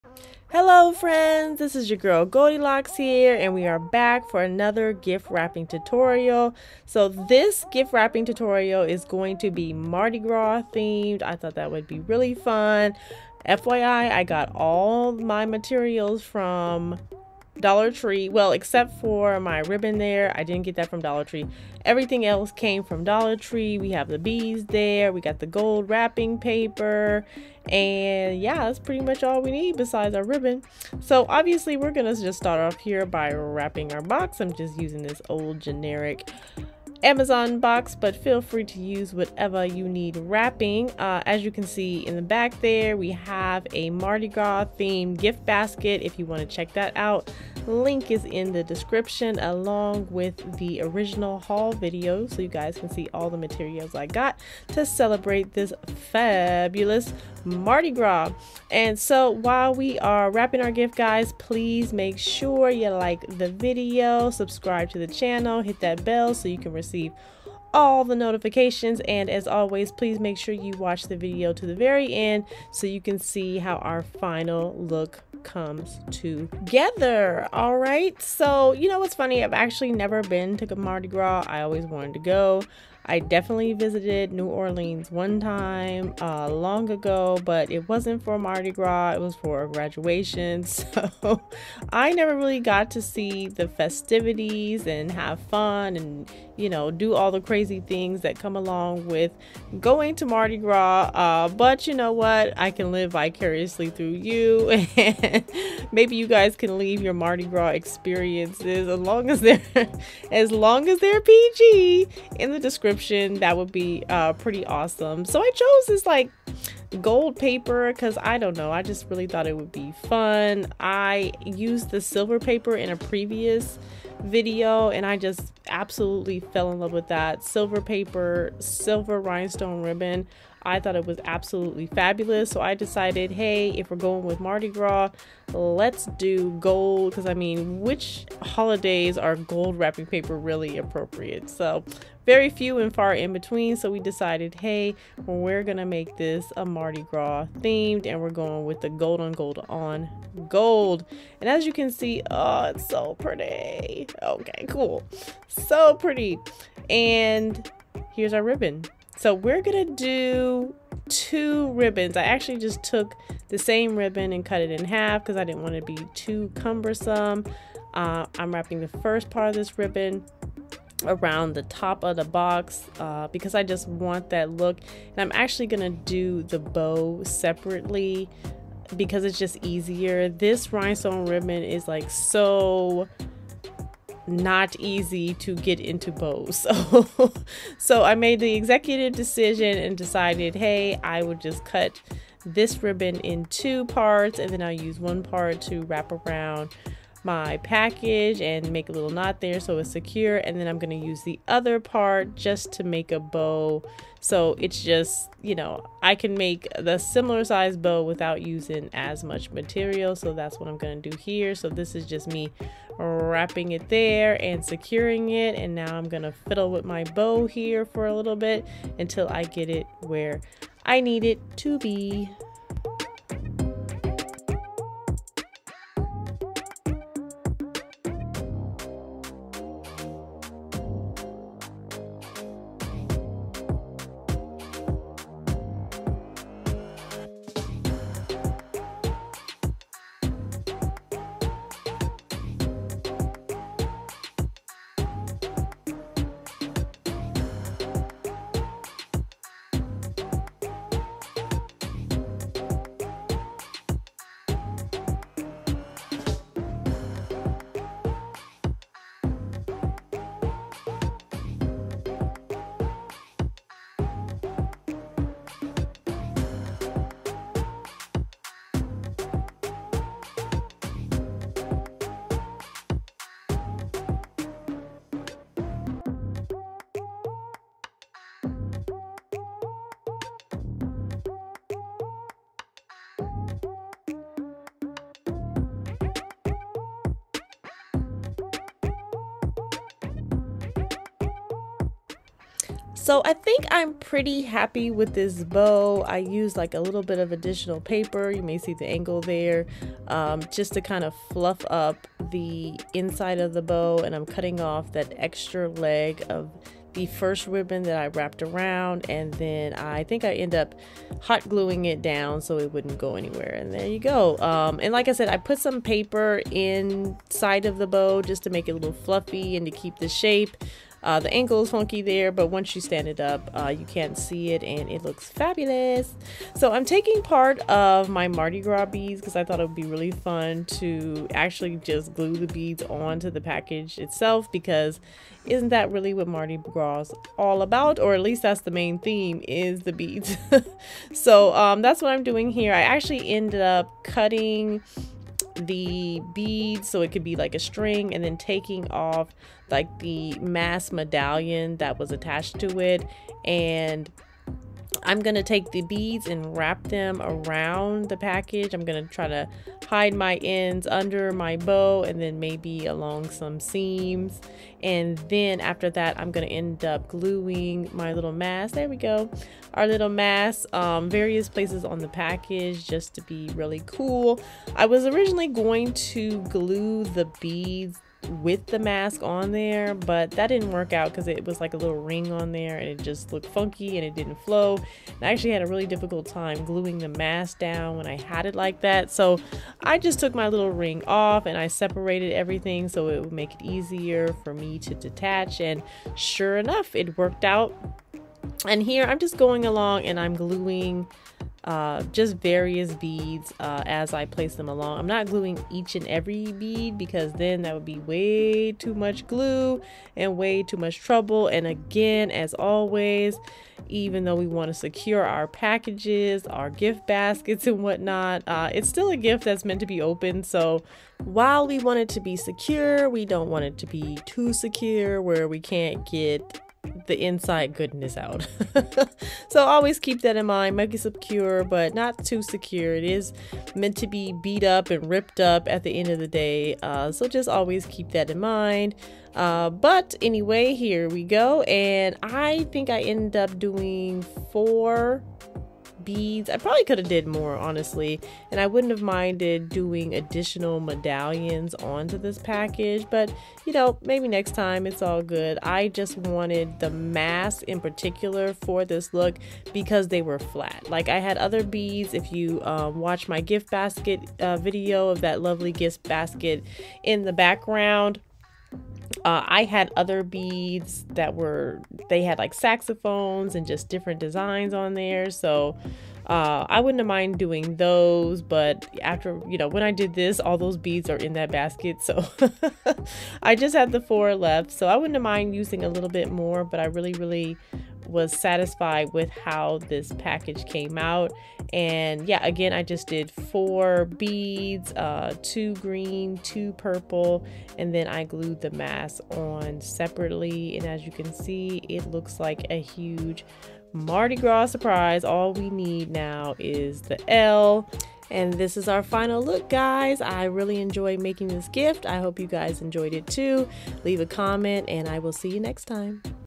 2023 Mardi Gras EASY & FUN DIY Gift Wrapping
Need a new way to celebrate your favorite party event? Mardi Gras, New Orleans will never be the same.

Gold Rhinestone Ribbon

🛒My Favorite Products: